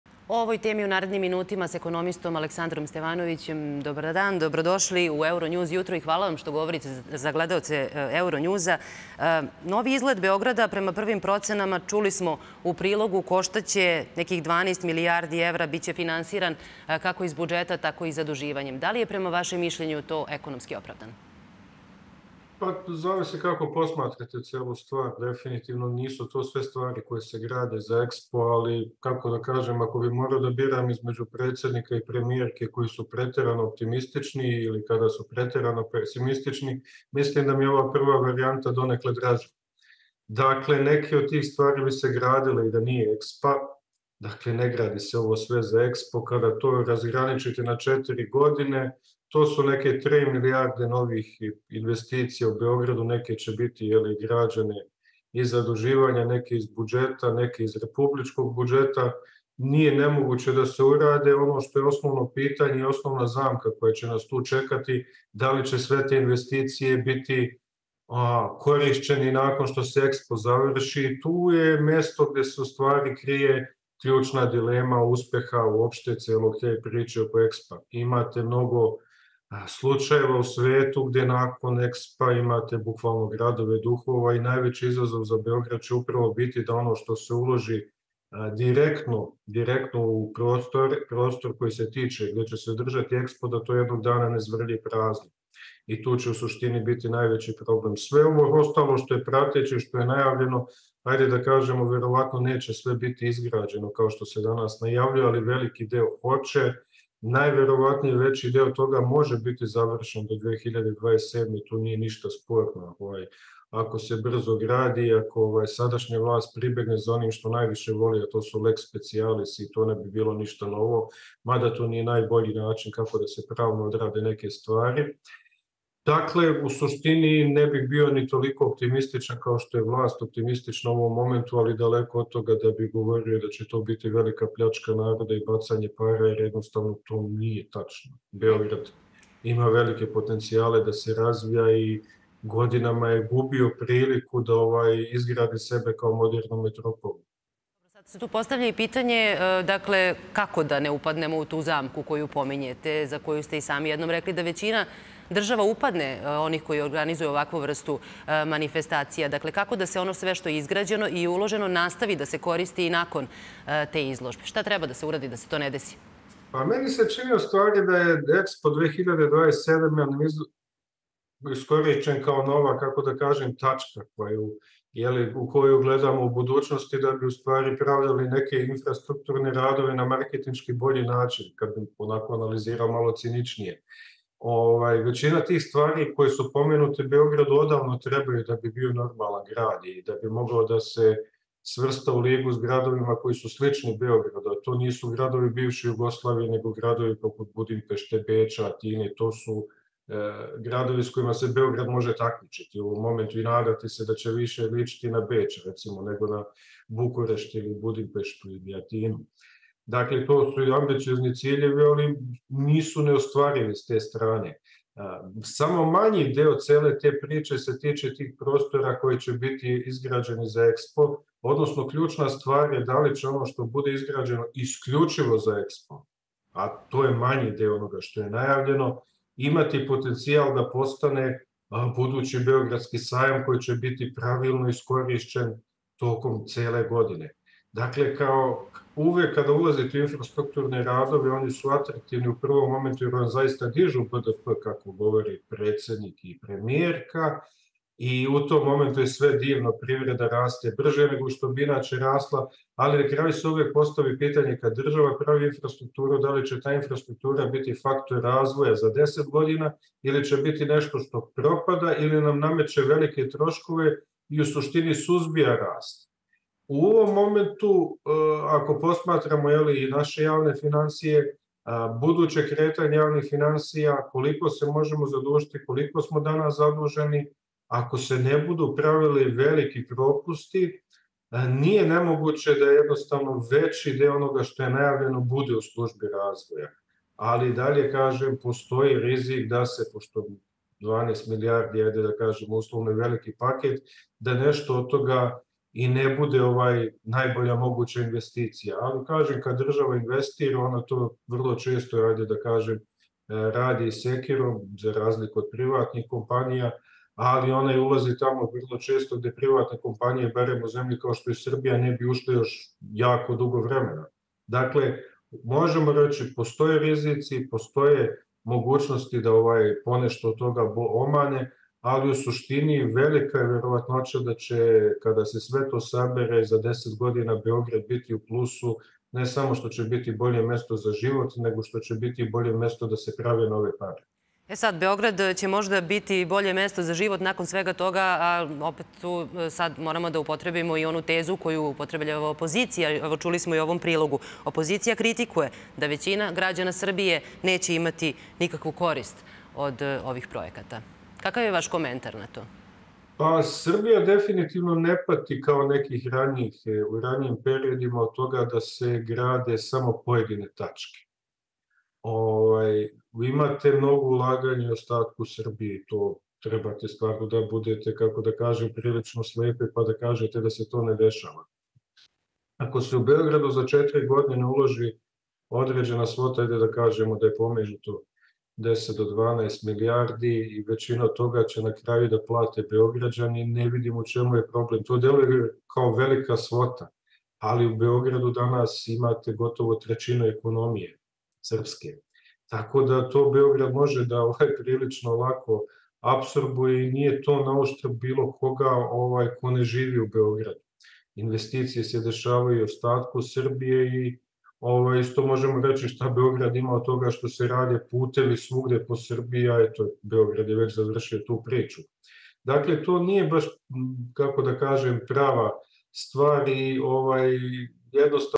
Stevanović: Ključno pitanje - da li će svi ti objekti koji se grade za EXPO biti korišćeni i kasnije
Postoji mnogo slučajeva  u svetu gde nakon manifestacije EXPO imate gradove duhova, tako da će najveći izazov za Beograd biti  upravo to da sve te investicije budu korisne i kasnije, kaže za Euronews Srbija ekonomista Aleksandar Stevanović. "Nisam optimističan u meri u kojoj vlast govori o tome, ali daleko od toga da mislim da će to biti velika pljačka naroda, kako kaže opozicija".